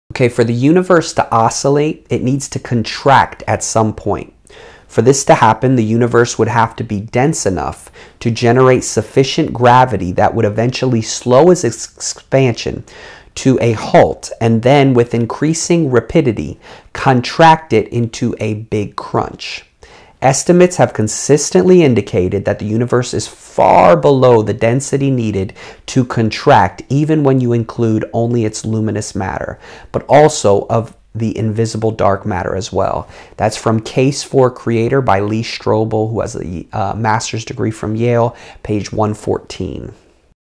Playing Card 172 - Why the Oscillating Model of the Universe Doesn't Work
WelcomeToTruth.com